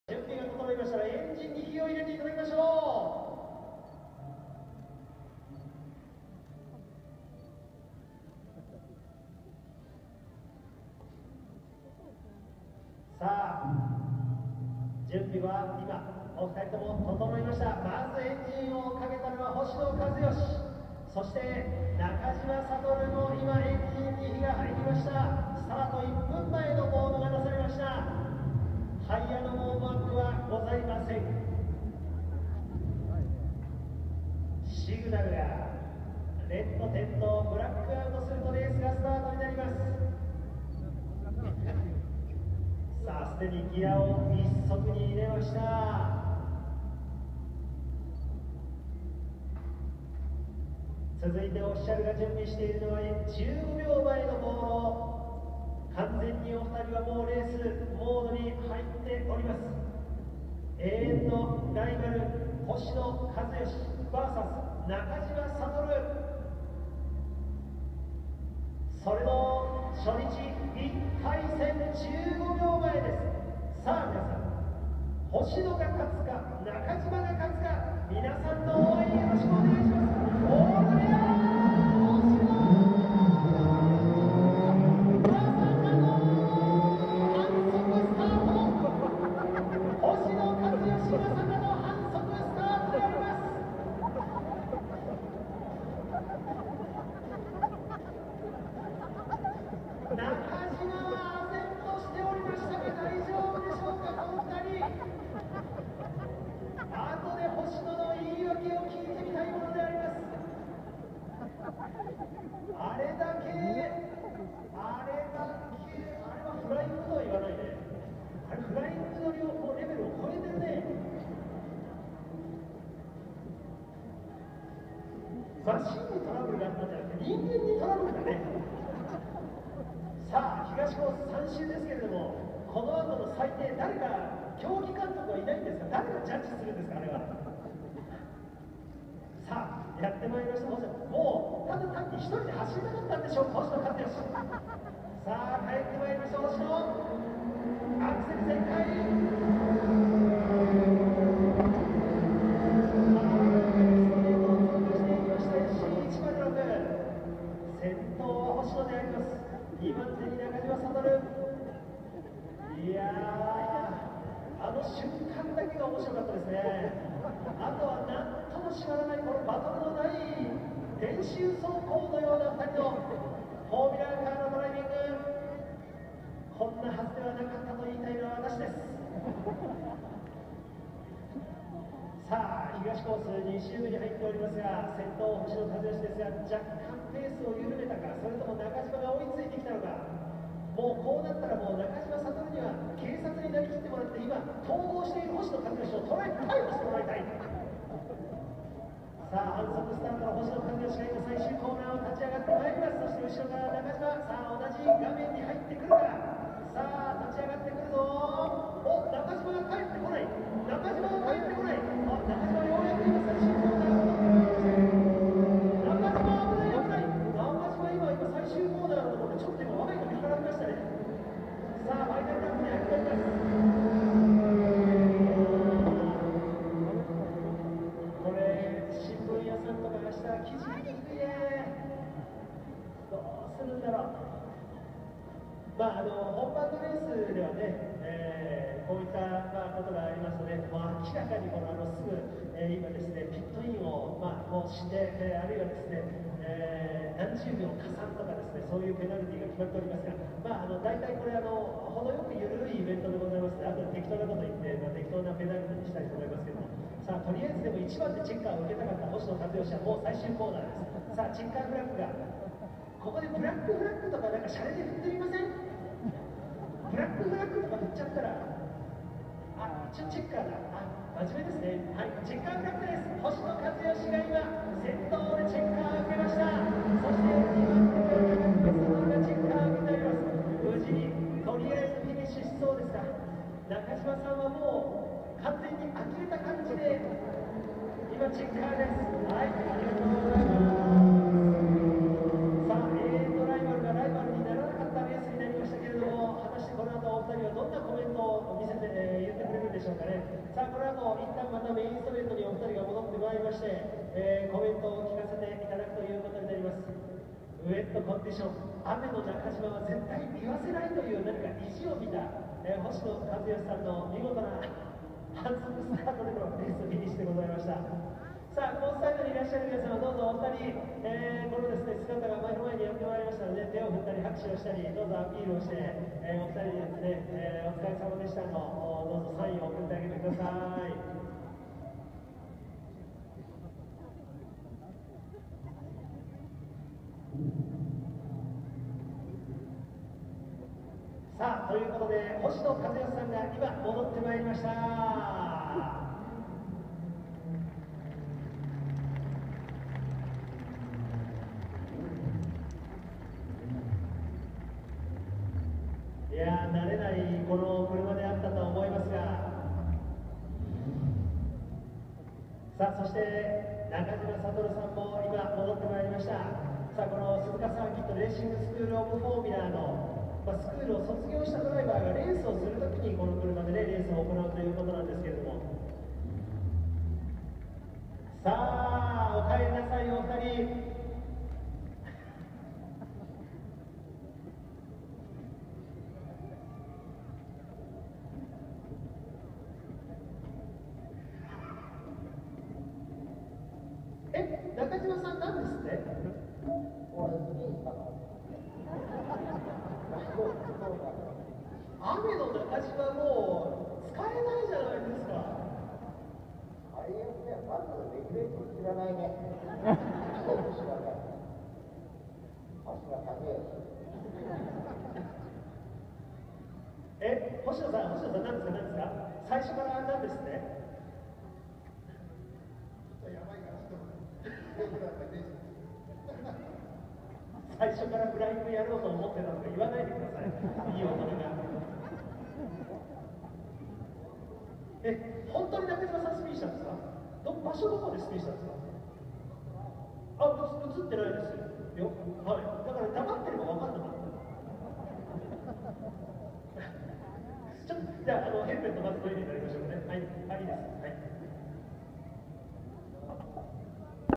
2015モータースポーツファン感謝デー、星野一義VS中嶋悟
2015.3.7 毎年恒例、永遠のライバル・星野一義VS中嶋悟の内容です。初心に戻るべく今年はレーシング受講生達の練習カーでもある車で対決しました。スタートランプが赤く光りブラックアウトするなりスタートすると思いきや超オーバーなフライングを星野一義が起こし、雨の中嶋悟と言われてましたがスピンをするといった波乱の展開でした（笑）